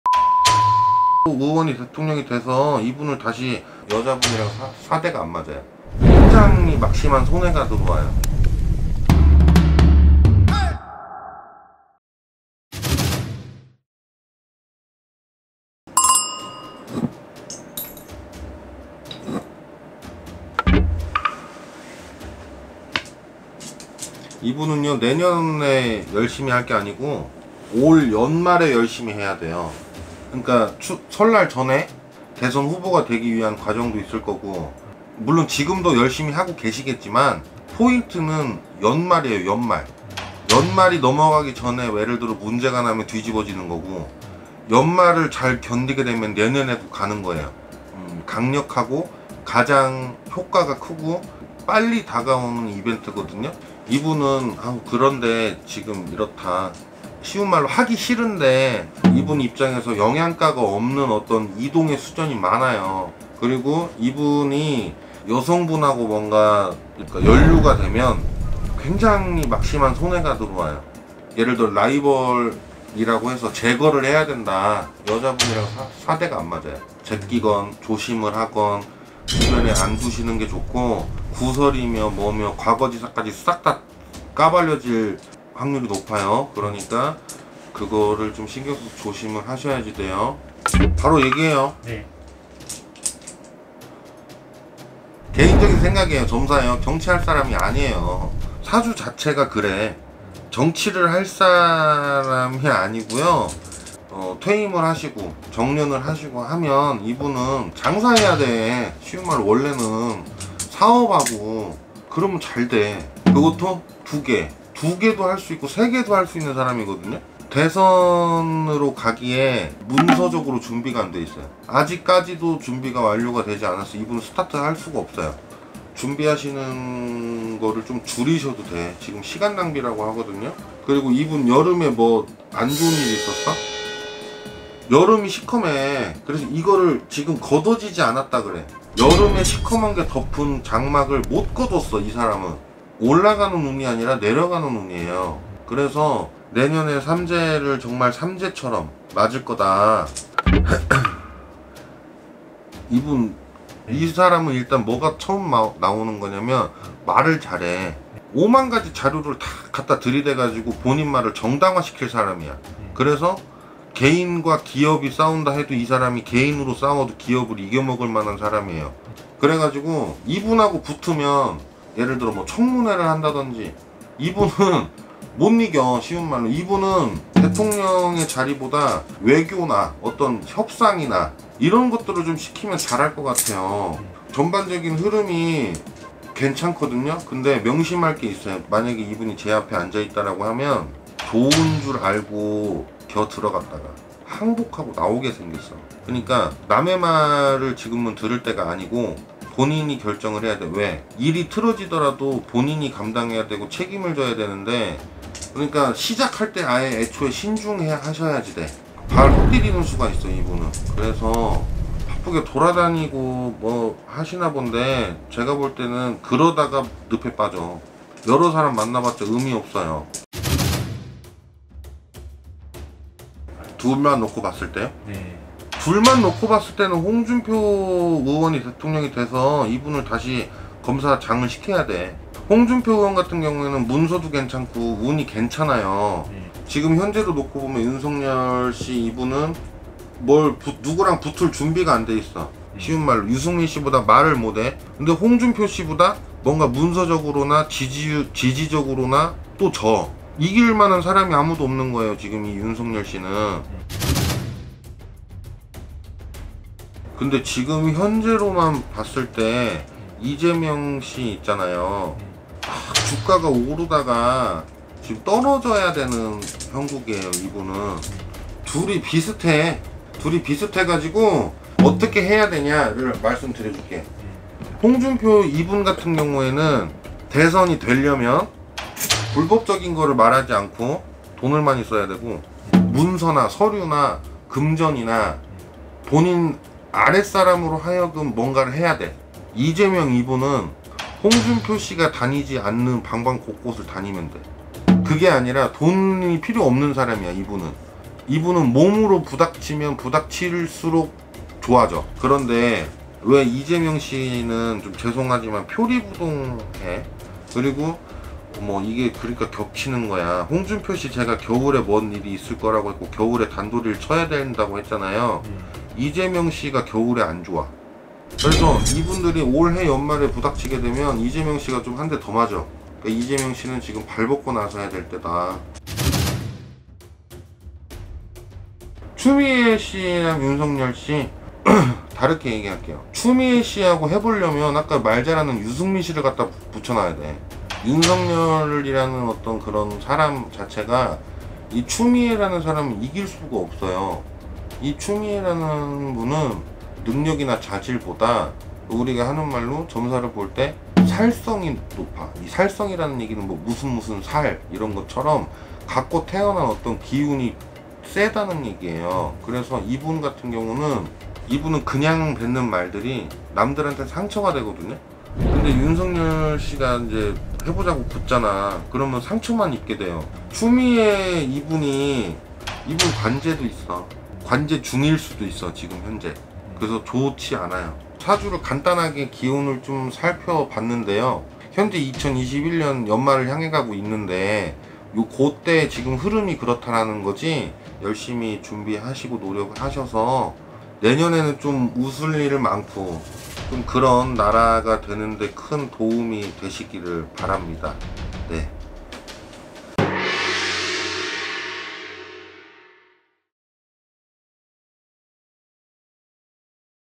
[출장도사] 무당이 본 국민의 힘 후보들의 예언! ··· 2022년 20대 차기 대통령은? 홍준표, 윤석열, 유승민 결국 모든 것이 들통난다?!!!
2022년 20대 대선 예언
국민의 힘, 예비 후보들의 치열한 공방
대선 경선 중 일어날 변화들은?

[ 지금 출장도사 앱을 설치하시고  천지도령 도사와 바로 상담해보세요! ]